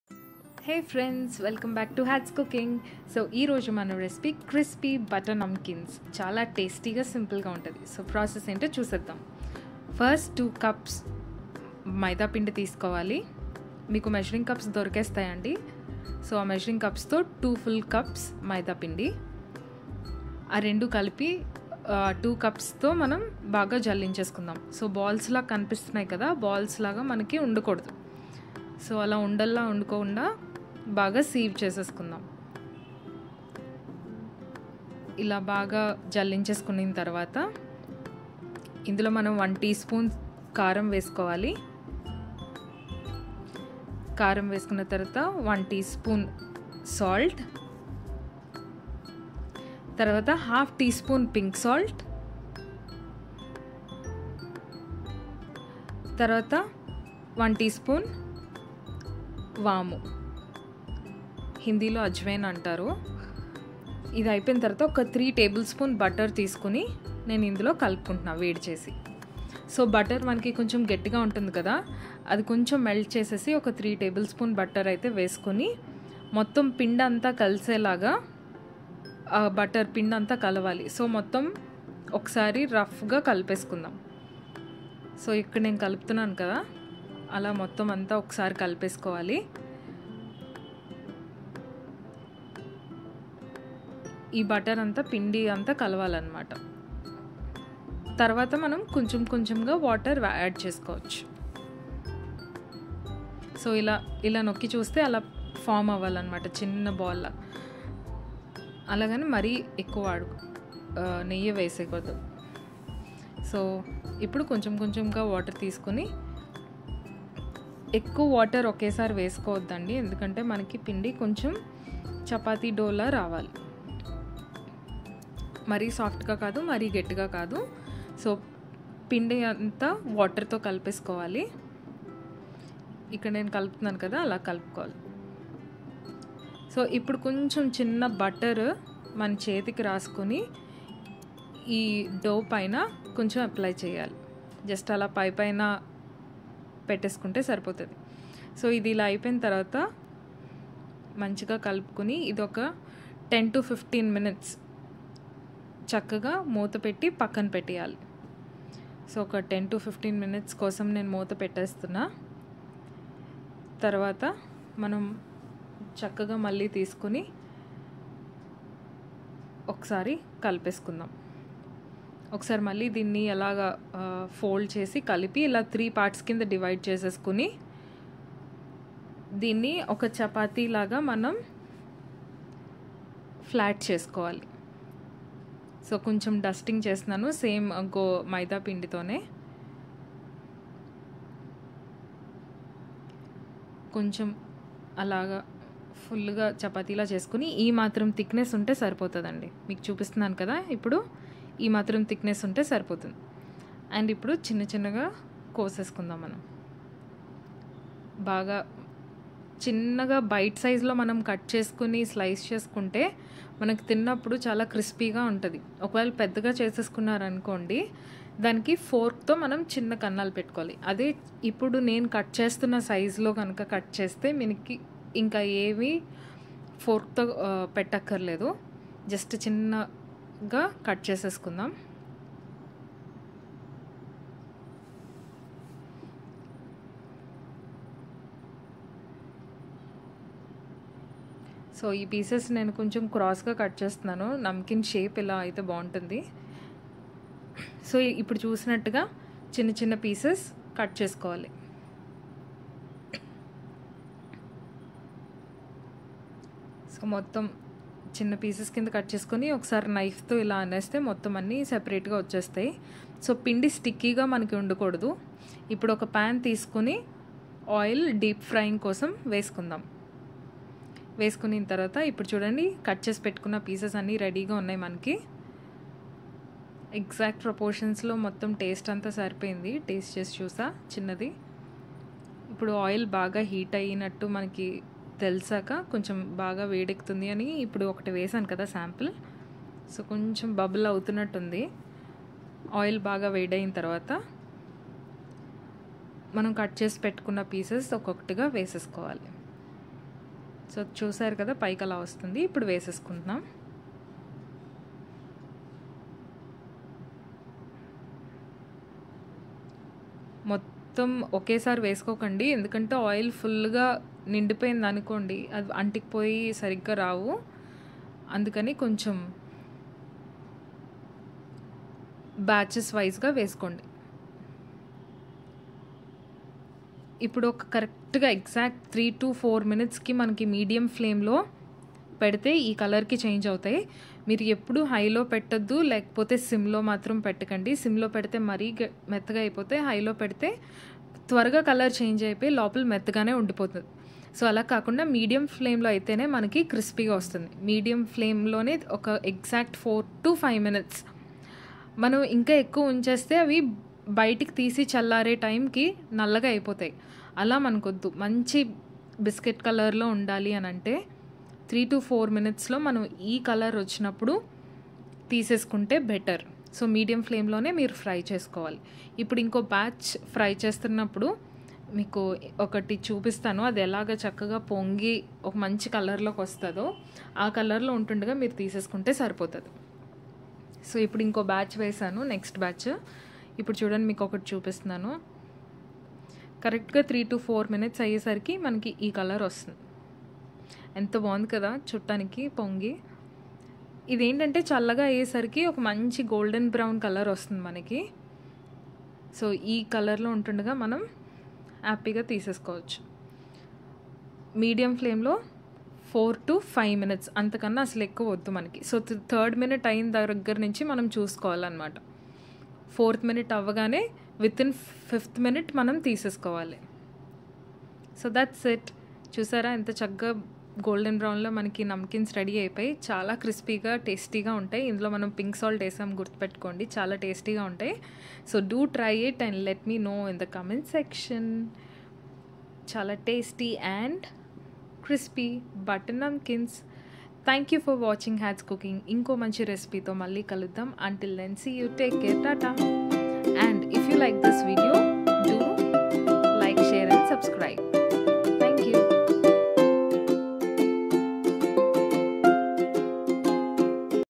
Namkeen Recipe// Crispy Butter Namkeen // Simple Snack// HC -19
Ingredients
2 Cups Maid
1 tspn Redchilli Powder
1/2 tspn Salt
1/2 tspn Pink Salt
1 tspn Ajwain/ Waamu
3 tbspns Melted Butter